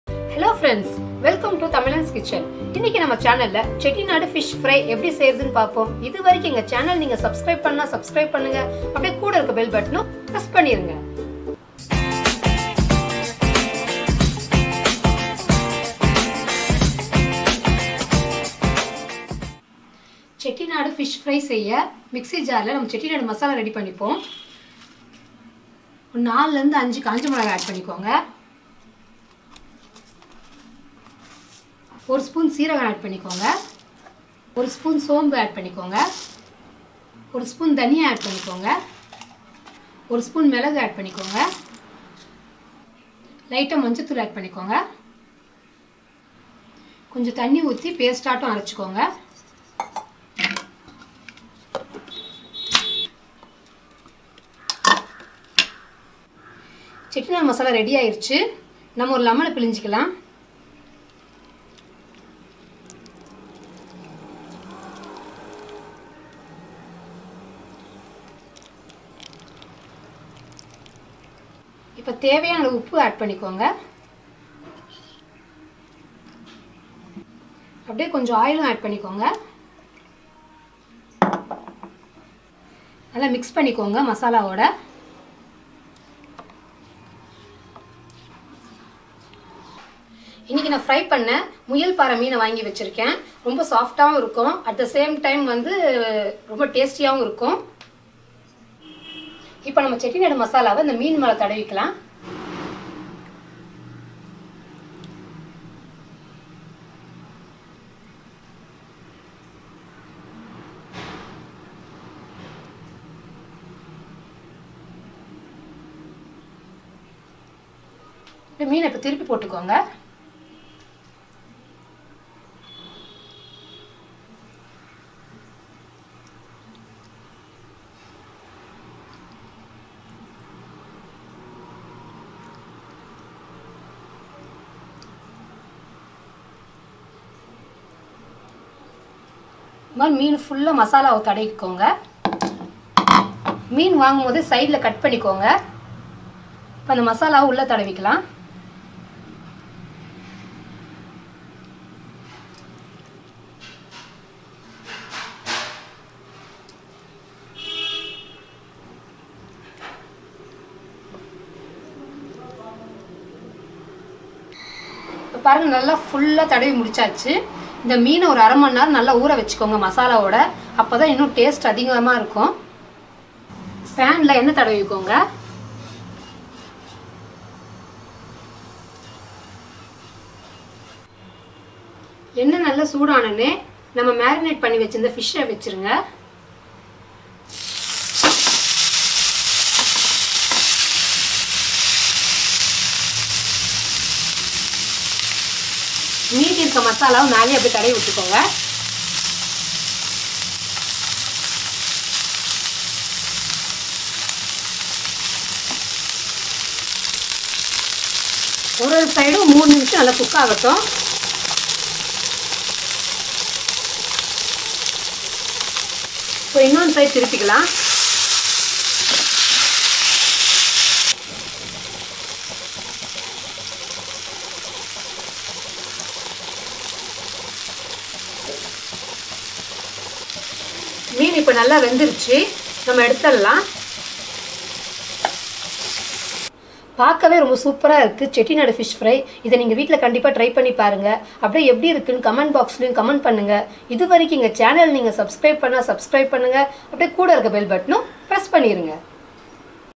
செட்டிநாடு மீன் வறுவல் | CHETTINADU FISH FRY | FISH FRY RECIPE IN TAMIL | TAMIZHAN'S KITCHEN
In this video I'm going to cook Chettinadu fish fry in tamil. It's a tasty,  spicy and authentic fish fry and tastes superb with Rice. Do try this recipe and leave your feedbacks in the comments section below.